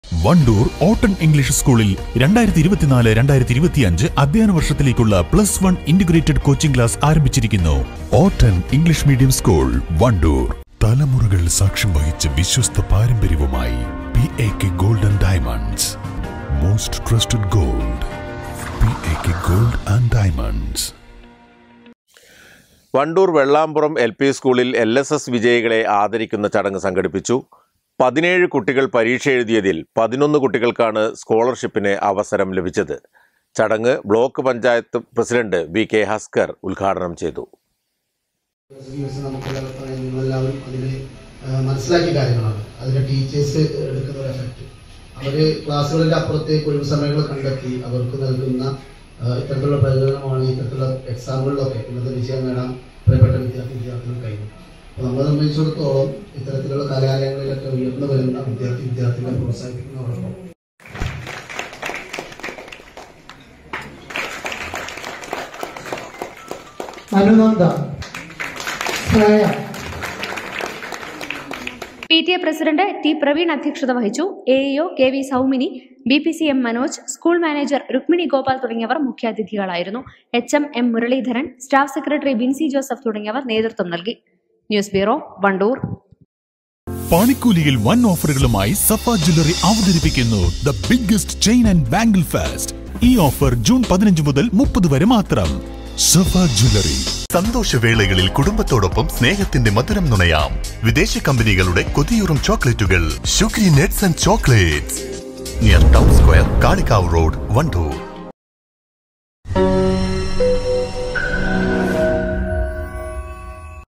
LSS വിജയികളെ ആദരിക്കുന്ന ചടങ്ങുമായി വെള്ളാമ്പുറം എൽ പി സ്കൂൾ
വി.കെ ഹസ്കർ ഉദ്ഘാടനം ചെയ്തു